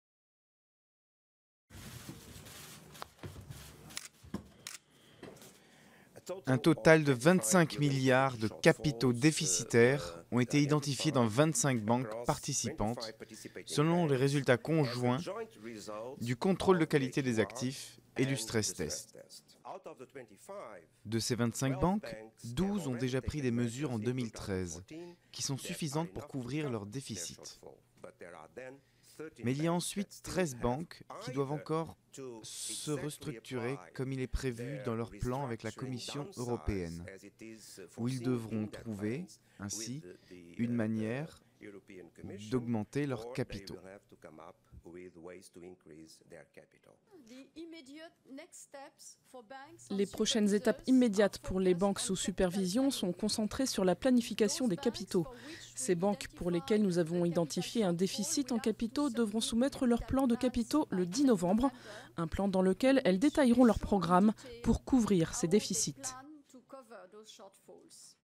25 banques de la zone euro ont échoué aux tests de résistance
Vingt-cinq banques de la zone euro, dont neuf italiennes, ont échoué à l'examen sans précédent mené par la Banque centrale européenne pour restaurer la confiance dans ce secteur-clé de l'économie, mais douze d'entre elles ont déjà pris des mesures pour corriger leur besoin en capital. Durée: 01:08